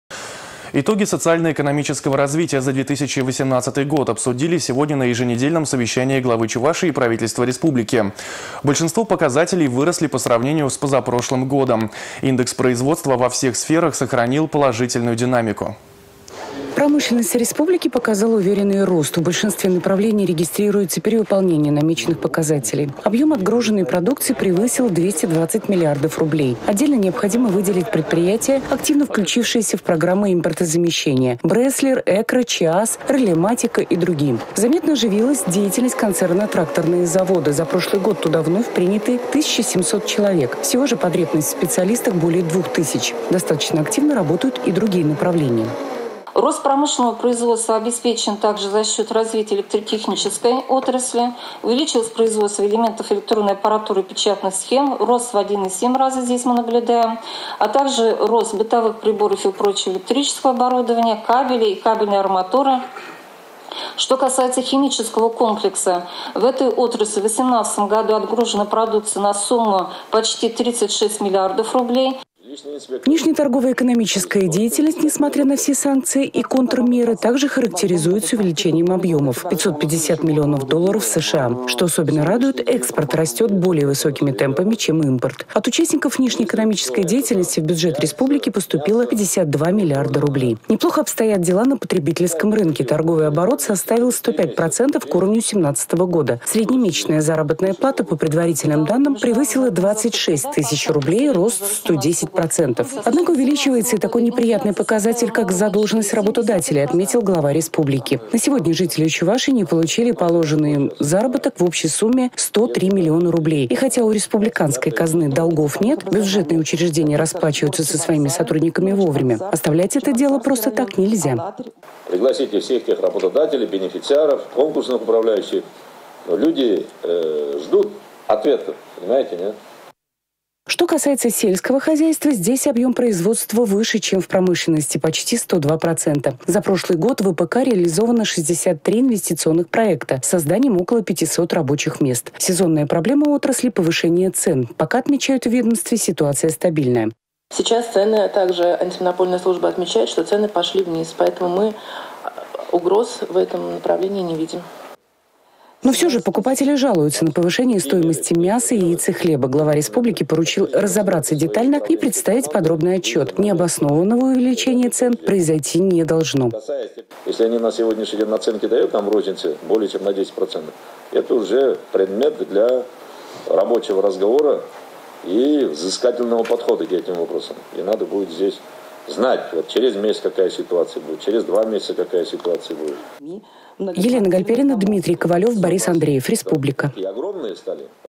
Итоги социально-экономического развития за 2018 год обсудили на еженедельном совещании
Большинство показателей выросли по сравнению с позапрошлым годом, индекс  производства во всех сферах сохранил положительную динамику.